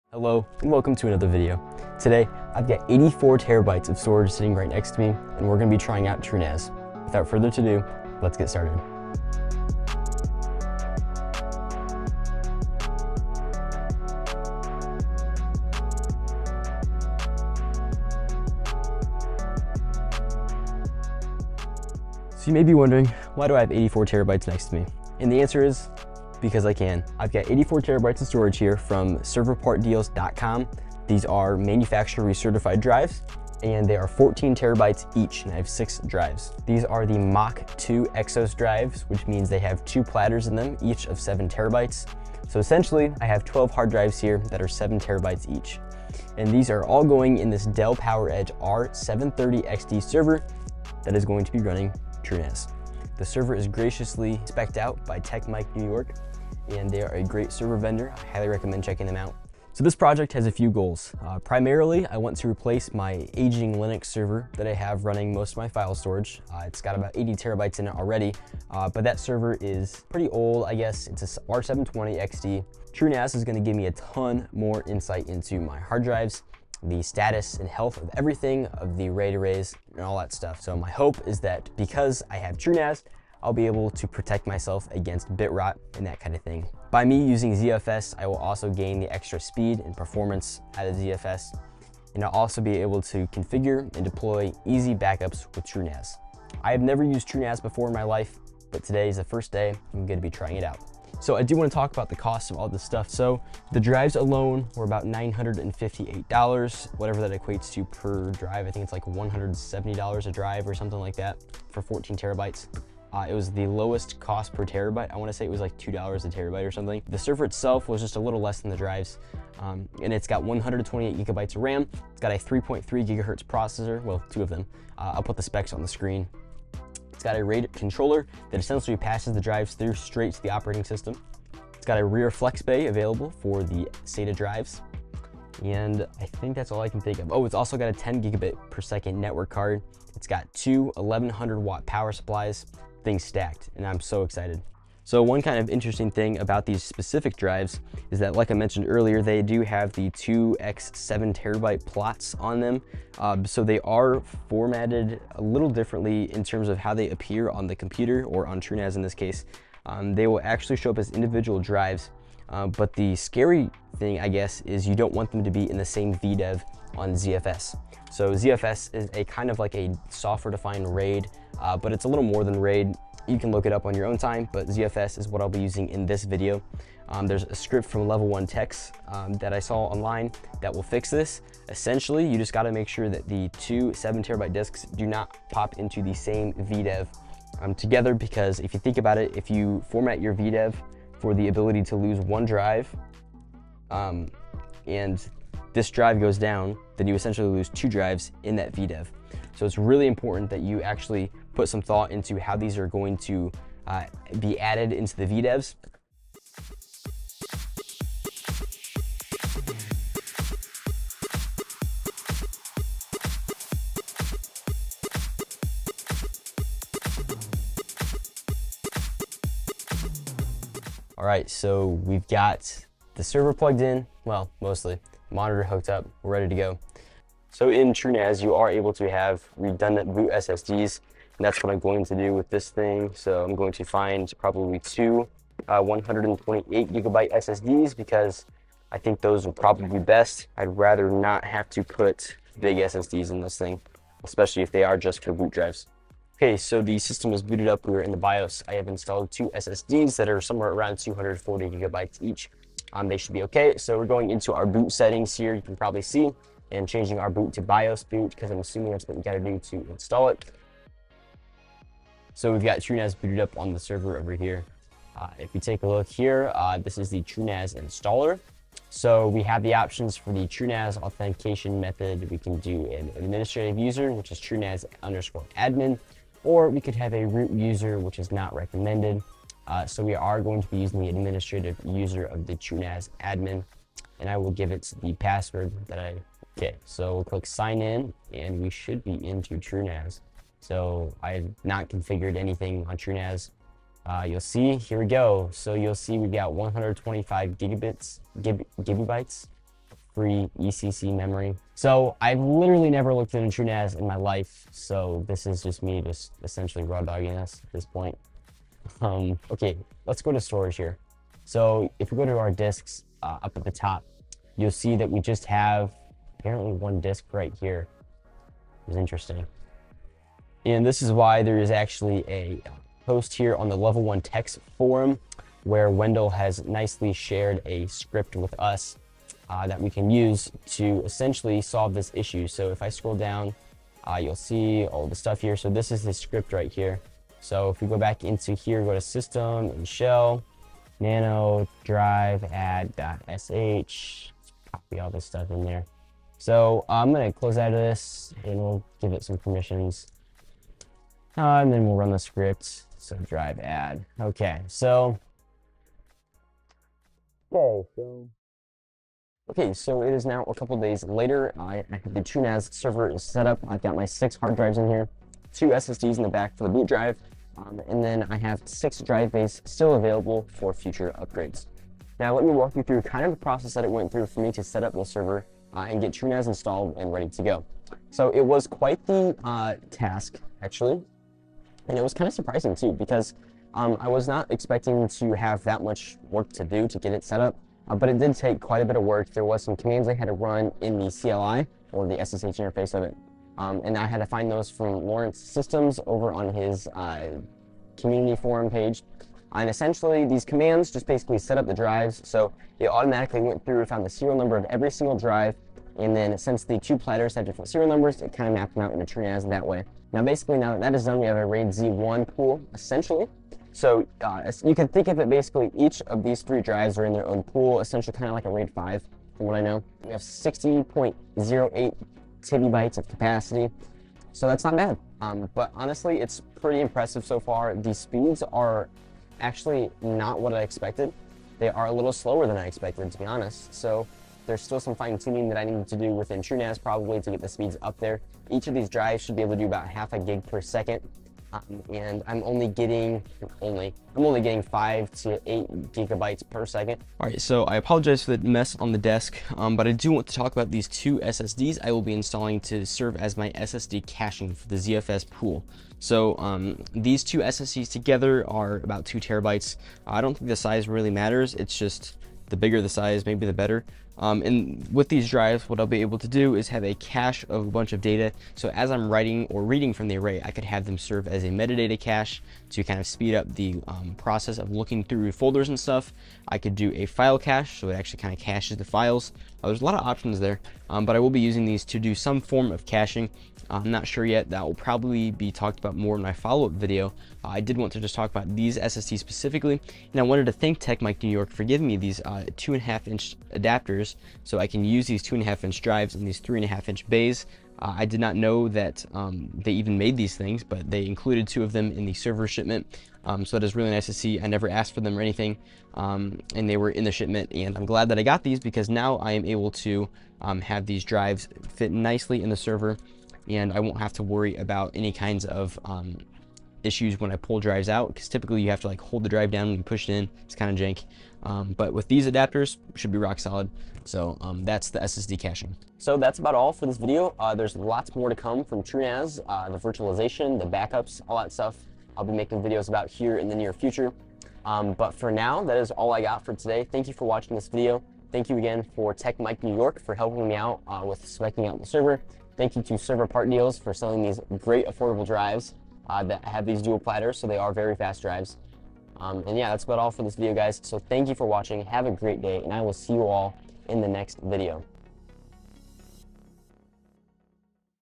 It's TrueNAS Time!
Forgot to mention in the video, but I am selling some servers! Check my community post for more information.

Hard drives were purchased from serverpartdeals.com

Timestamps:
0:00 - Intro
0:26 - Parts
0:59 - Goals
1:25 - ZFS
1:40 - Cost
2:24 - ZFS on these drives
3:47 - Redundant Boot SSDs
4:22 - Trying to setup the pool
5:09 - Learning TrueNAS?
5:20 - Getting help
5:55 - This is hard
7:05 - Slow speeds
7:27 - SSD Caching
8:45 - Conclusion

01152025